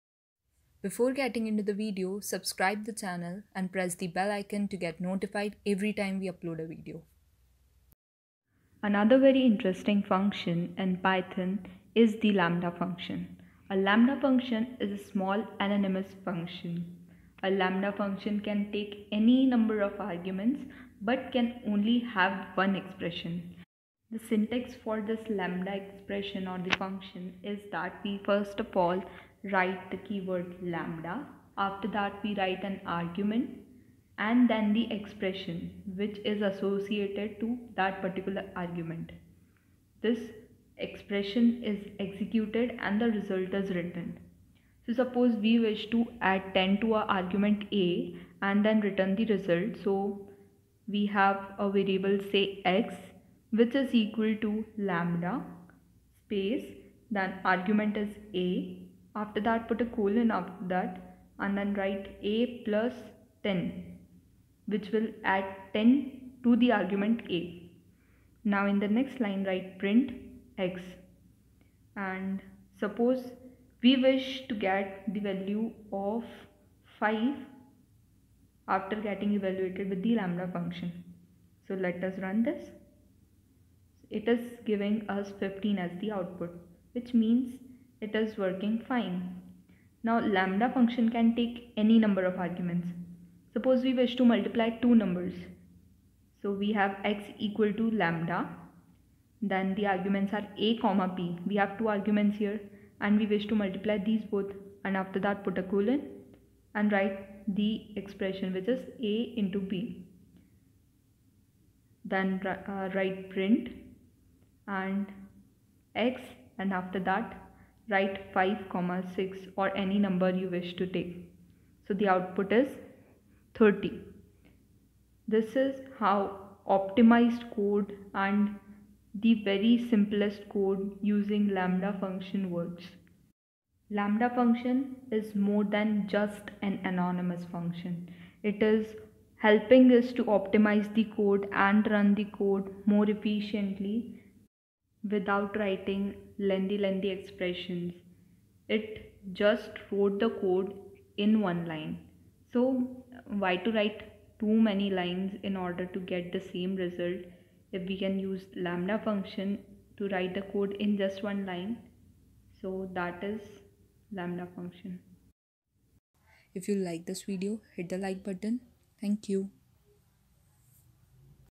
Lambda Function: Tutorial 33
A lambda function is a small anonymous function. It can take any number of arguments, but can only have one expression.

Example: Add 10 to argument a, and return the result:

x = lambda a : a + 10
print(x(5))

Lambda functions can take any number of arguments.

Example: Multiply argument a with argument b and return the result:

x = lambda a, b : a * b
print(x(5, 6))